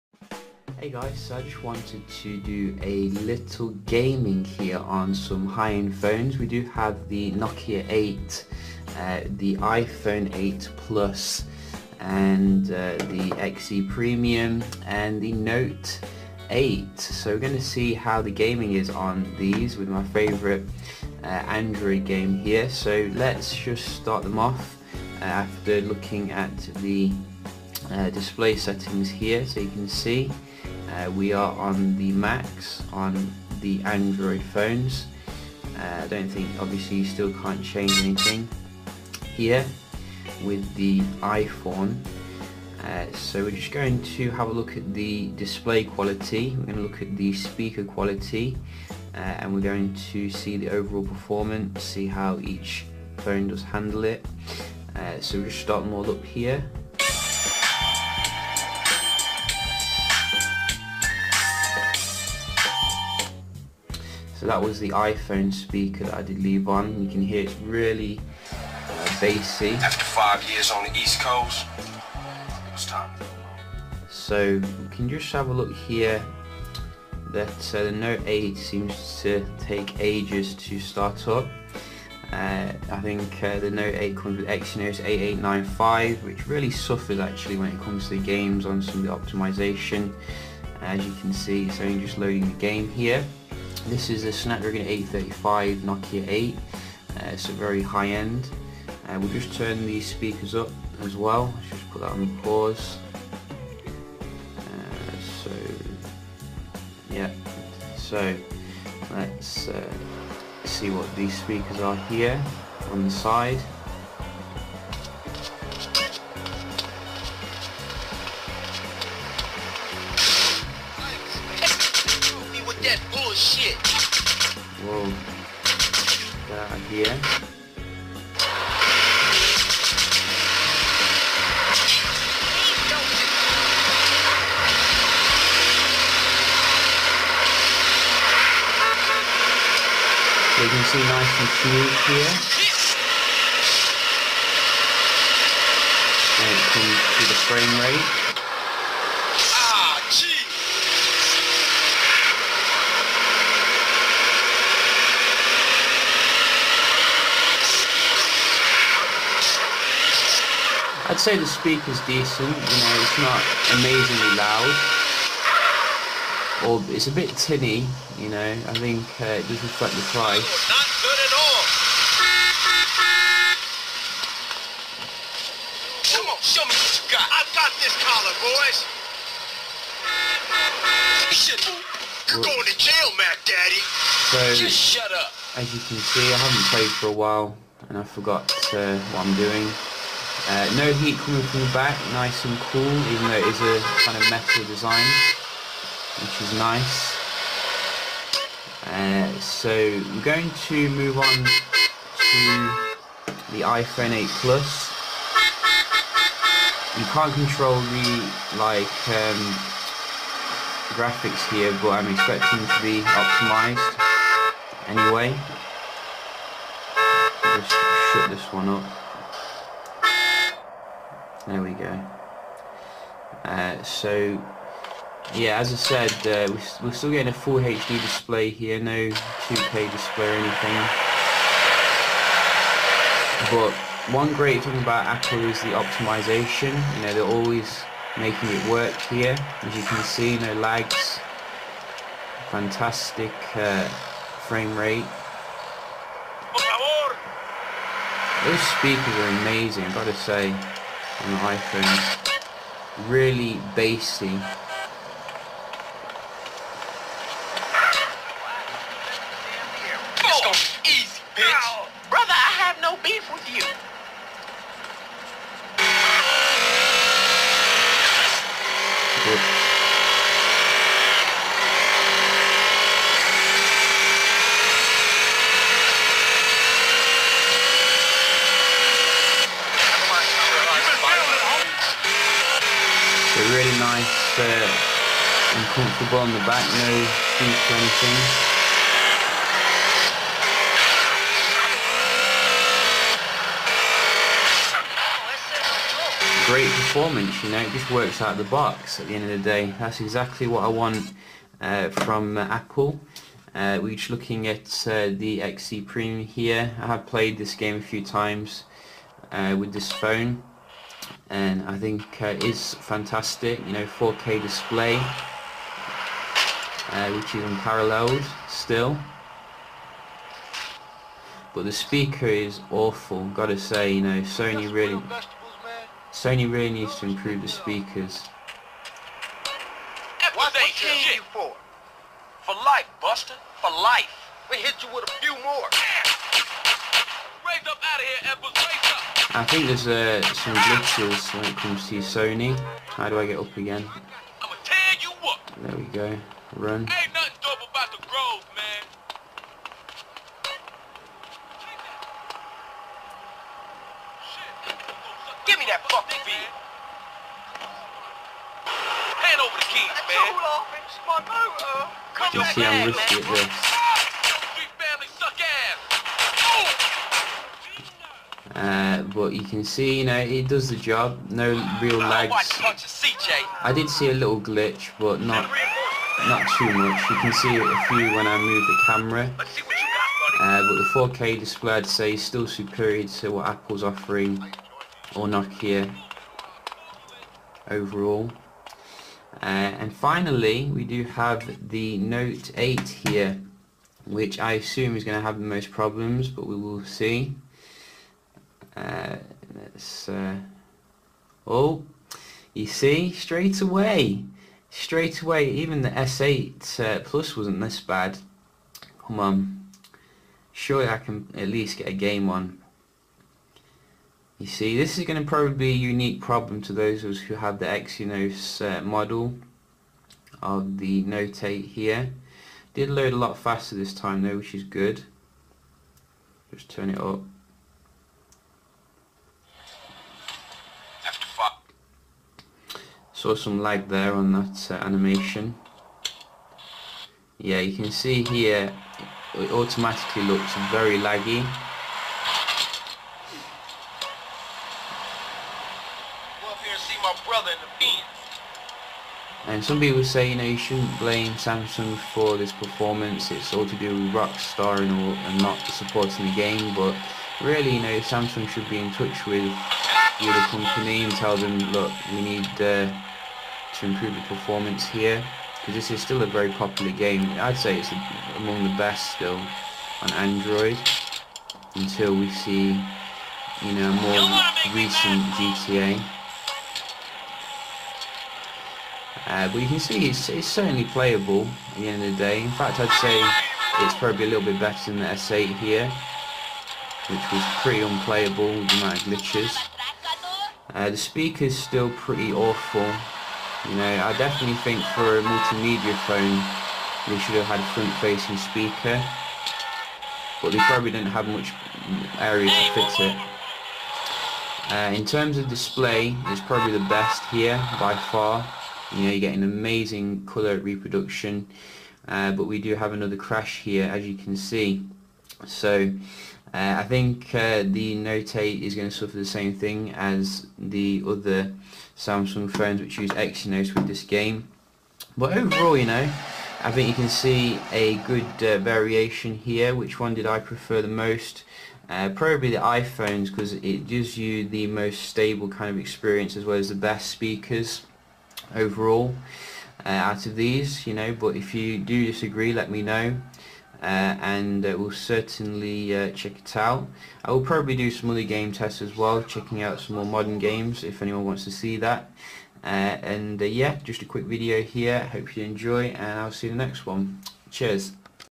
iPhone 8 Plus vs Note 8 vs Nokia 8 vs Xperia XZ Premium - Gaming Comparison/Gaming Test!
All the 8's come together for a gaming comparison, iPhone 8 Plus, Nokia 8, Note 8, Xperia XZ Premium.